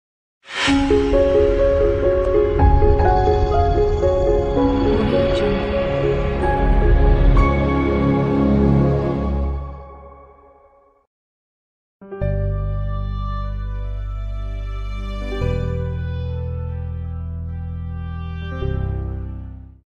Go Downstairs Barefoot to The Shop
The model went out barefoot， took the elevator downstairs， went shopping in the supermarket along the street， and then returned home.